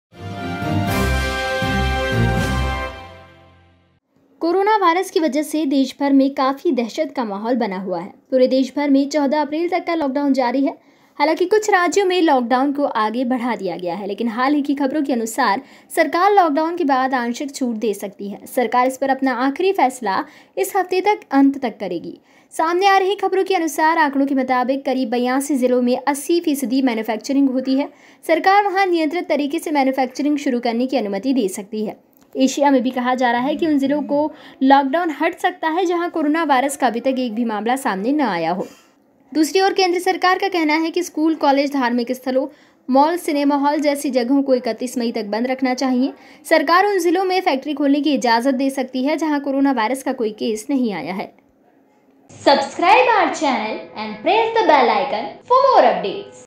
Corona Lockdown : 14 April के बाद इन जिलों को Lockdown से मिल सकती है राहत | Ghamasan Live
Corona Lockdown : 14 April के बाद इन जिलों को लॉकडाउन से मिल सकती है राहत | Ghamasan Live

कुछ राज्यों में लॉकडाउन को आगे बढ़ा दिया गया
सरकार लॉकडाउन के बाद आंशिक छूट दे सकती है
14 अप्रैल के बाद लॉकडाउन से मिल सकती है राहत